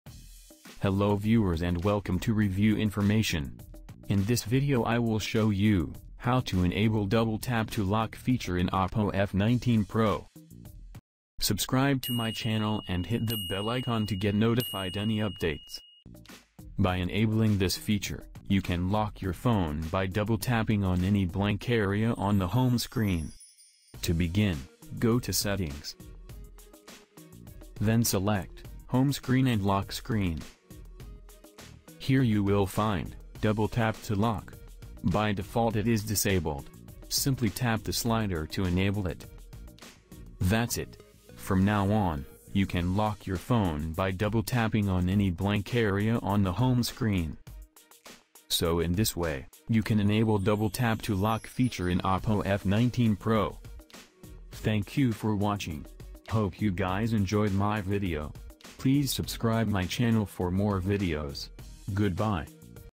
How to Enable Double-Tap to Lock Feature in Oppo F19 Pro
In this video I will show you, How to Enable Double-Tap to Lock Feature in Oppo F19 Pro. By enabling this feature, you can lock your phone by double tapping on any blank area on the home screen.

Go to settings. Select home screen & lock screen. Here you will find, double-tap to lock. By default it is disabled. Simply tap the slider to enable it. That's it. From now on, you can lock your phone by double tapping on any blank area on the home screen.